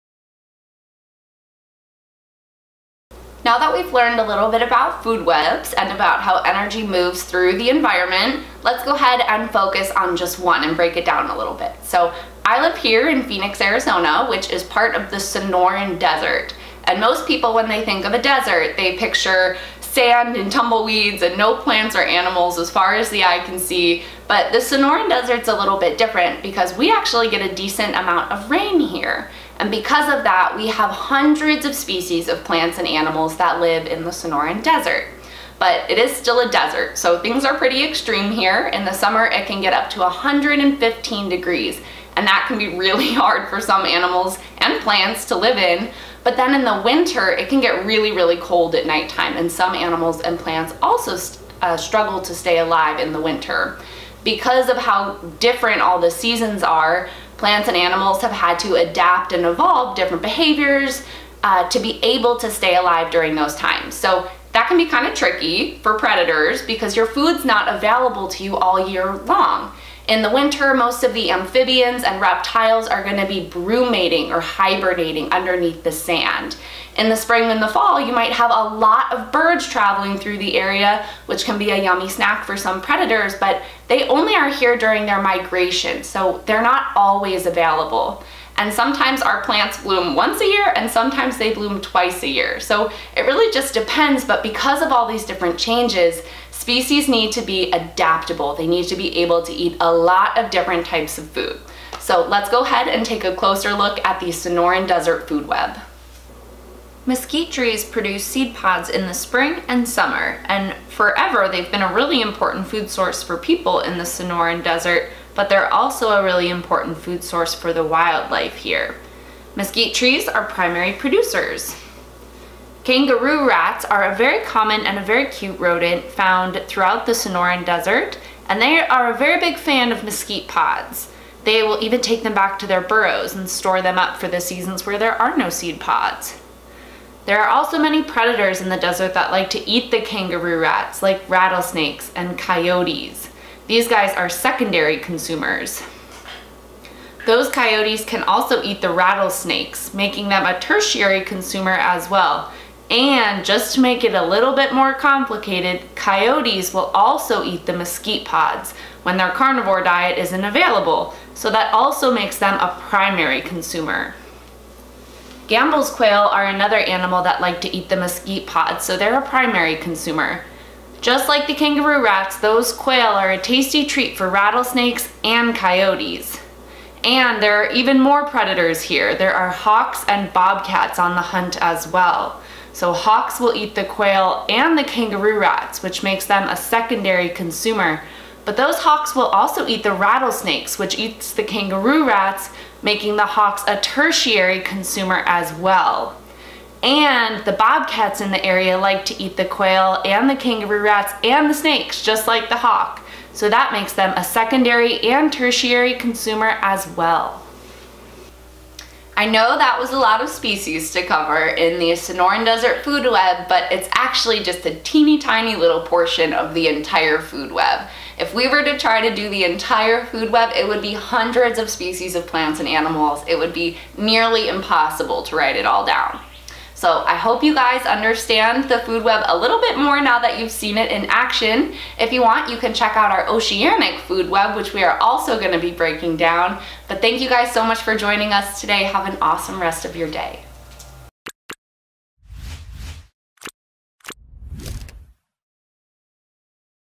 Desert Food Webs | Desert Food Web Examples
Future Zoologist Academy is a virtual zoology and ecology program for kids who love animals.

A lot of people picture a barren, empty landscape with dust and tumbleweeds when they picture a desert. And while that may be true for some deserts, it is certainly not for the Sonoran Desert. The Sonoran Desert is a relatively moist desert in the Southwestern United States and Mexico. Sometimes receiving up to 20 inches of rain in a single year, the Sonoran Desert is filled with hundreds of well-adapted plant and animal species which decorate the beautiful landscape and create a bustling ecosystem. The intense and extreme seasons have caused many species to thrive at various points throughout the year, which means food availability for consumers is ever-changing. Plentiful mesquite trees produce seeds pods in the spring which is a popular energy source for many species. Many snakes and lizards sleep in burrows for most of the winter out of reach from predators. Different predatory birds move throughout the ecosystem during their migration seasons. With the wide variety of organisms and seasonality of life, the Sonoran Desert is home to many intricate food webs and species which must have a flexible and expansive diet. Let’s break one down!

-Mesquite trees produce seed pods in the spring and summer and have historically been a very important food source for people in the Sonoran Desert, and even more important for the vast variety of wildlife. They are a primary producer.

-Kangaroo rats are a common (and adorable) rodent found throughout the desert and a very big fan of mesquite pods, making them a primary consumer. They will even take them back to their burrows to store them for the other seasons.

-There are many predators in the desert that like to eat kangaroo rats, like rattlesnakes and coyotes, making them secondary consumers. Coyotes also like to eat the mesquite pods when their carnivore diet isn’t as available, making them a primary consumer, as well!

-Gambel’s quail also like to eat the mesquite pods, making them a primary consumer. Like the kangaroo rats, quail are also a tasty treat for rattlesnakes and coyotes.

-There are even a few more predators in the area. Hawks and bobcats are on the hunt. Hawks will eat the rattlesnakes which eat the quail and kangaroo rat, making them a secondary consumer. Hawks will also eat the rattlesnake which eats the kangaroo rat, making them a tertiary consumer, too!

-Bobcats in the area like to eat the quail, kangaroo rats, and the snake, just like the hawk, making them a secondary and tertiary consumer, also!

This is by no means the entire food web of the Sonoran Desert, just a small example.